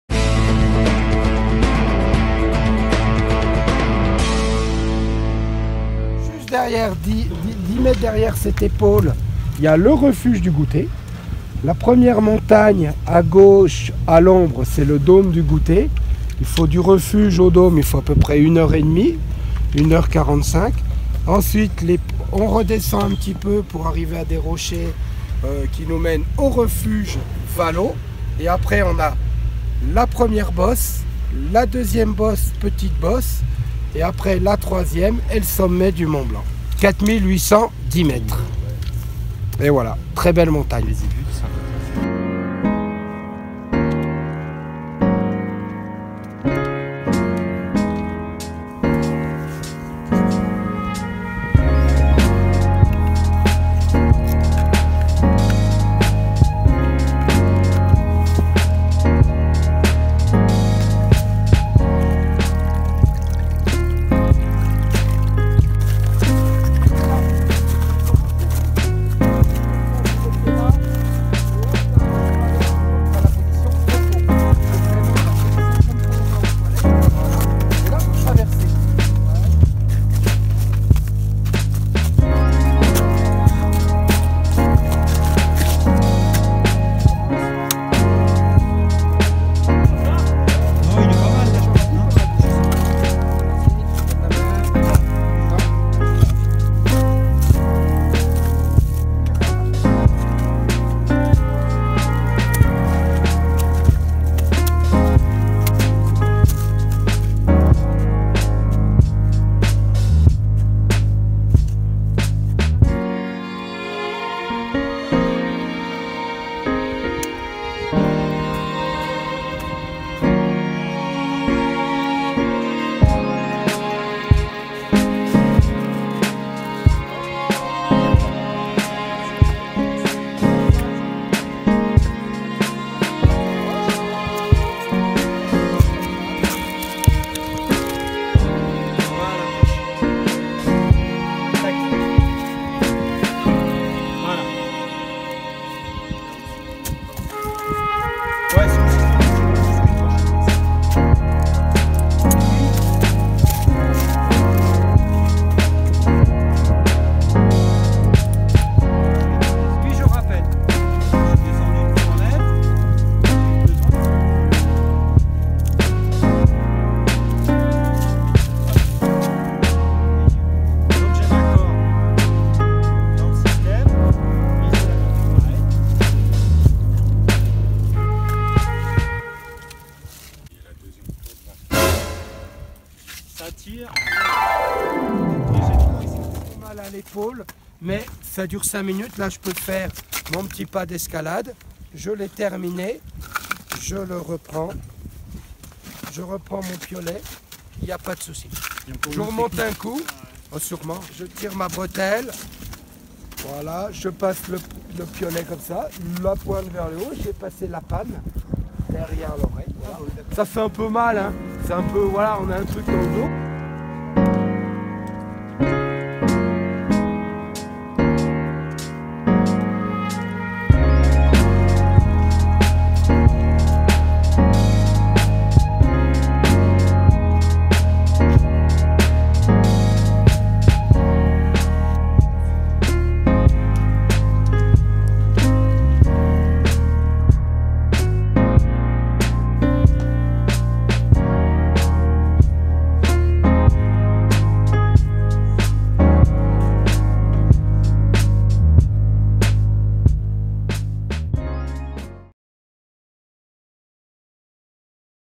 Ecole de Glace, le 29 août 2018
Ecole de Glace au Glacier du Tour, première des trois journées de préparation à l'ascension du Mont Blanc.